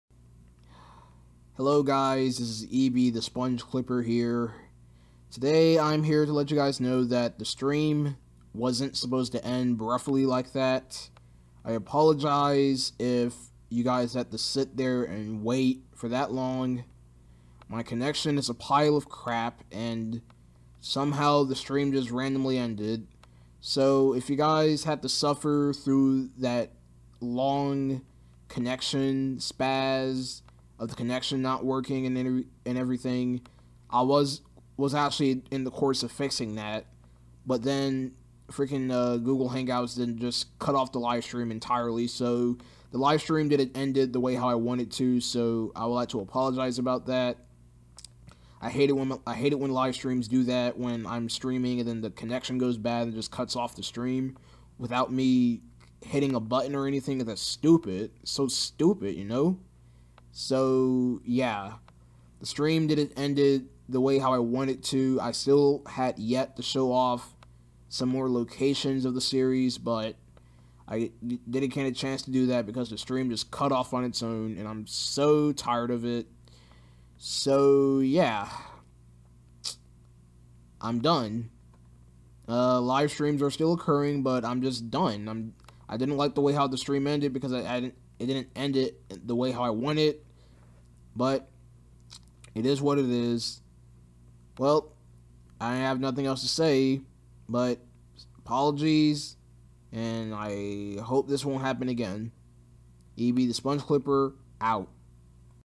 A Huge Apology to The Ending of The Finale Stream
The live stream wasn't meant to end guys. My connection was really bad, I tried to fix it but Hangouts just decided to cut off the stream. A huge apology for the stream not having a proper ending.